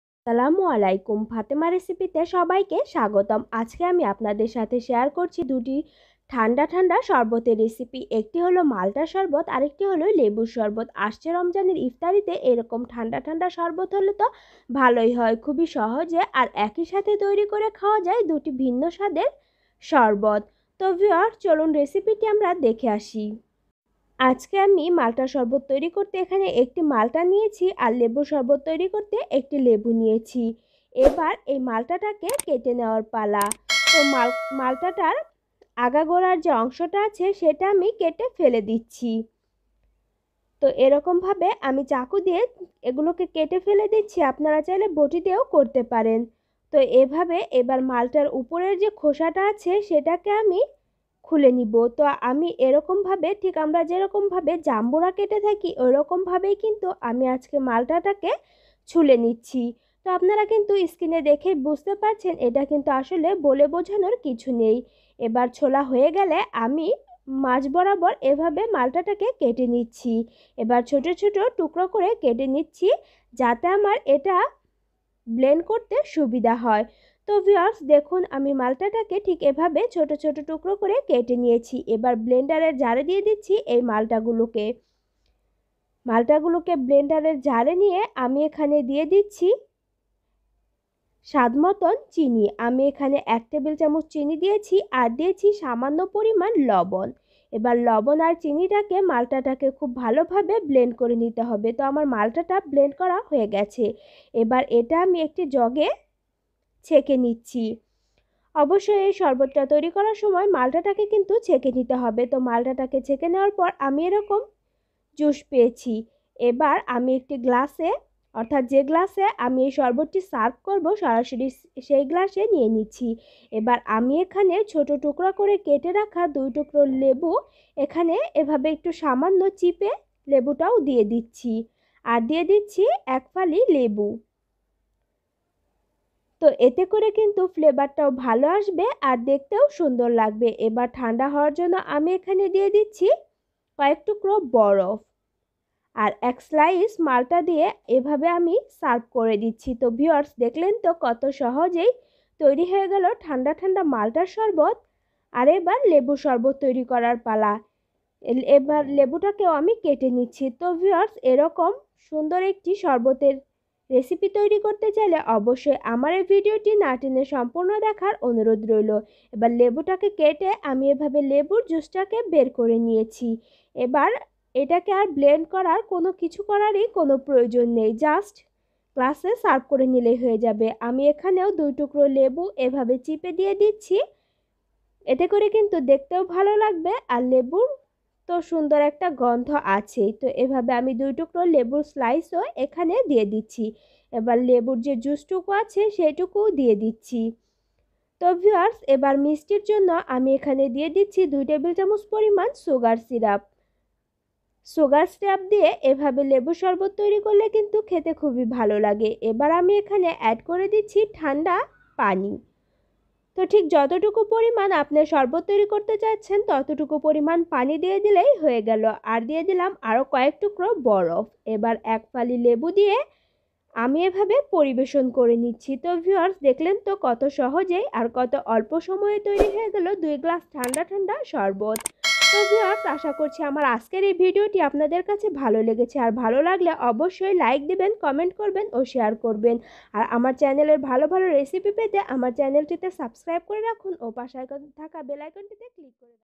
২ গ্লাস ঠান্ডা ঠান্ডা সরবত || লেবু মাল্টার সরবত (রমজান স্পেশাল) / Cold Cold Lemon & Maltar Juice
আসছে রমজানের ইফতারিতে যদি এ রকম ঠান্ডা ঠান্ডা সরবত হয় তাহলে তো সারাদিনের ক্লান্তি এক নিমিষেই দূর হয়ে যায়। তাই আজ আপনাদের সাথে শেয়ার করছি খুবই সহজ আর অল্প সময়ে কিভাবে তৈরি করা যায় ২ ধরনের জুস সেই রেসিপিটিই।

আমার অন্যান্য সামাজিক যোগাযোগ মাধ্যম-